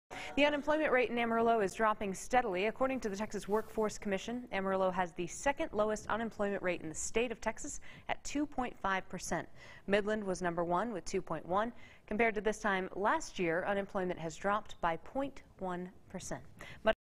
Amarillo Has 2nd Lowest Unemployment Rate in Texas
The unemployment rate in Amarillo is steadily dropping.